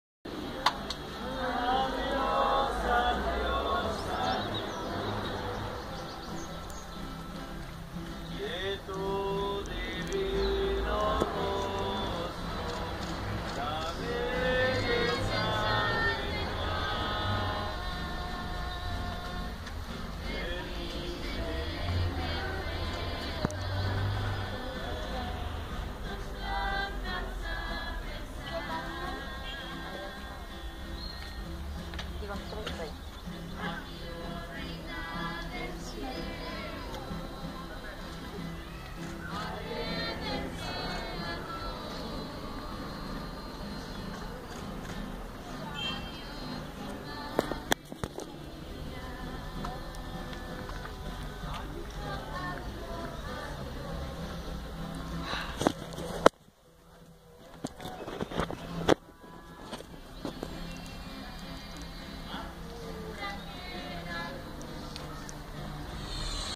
Despiden dolientes a familia atropellada por patrulla en Córdoba
Los restos de ambos fueron despedidos por sus dolientes en un panteón local.